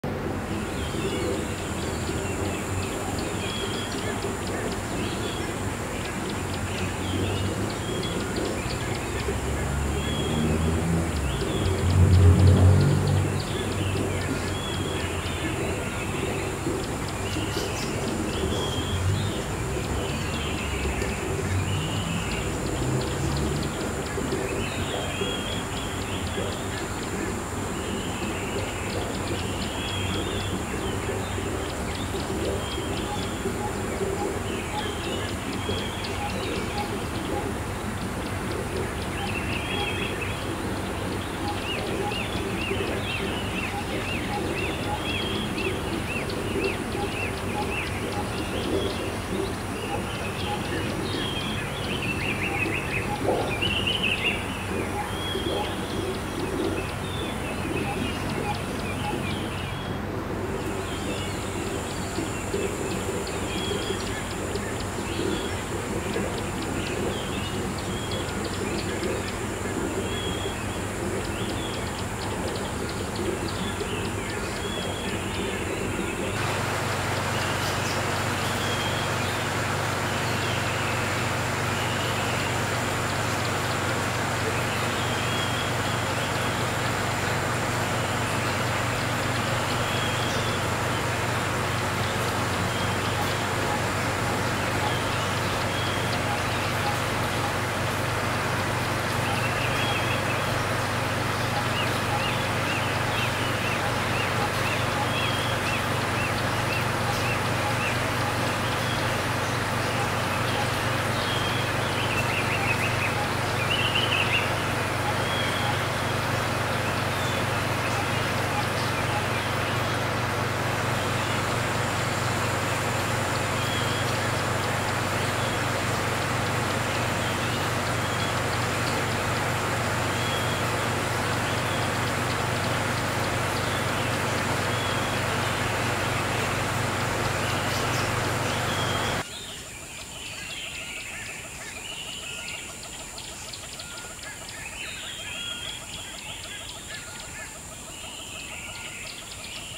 Giant Aimara wolf fish with a hint of Channa Auranti.....
Fishroom progress update for the Aimara wolf fish and the newly acquired wild caught Channa Auranti.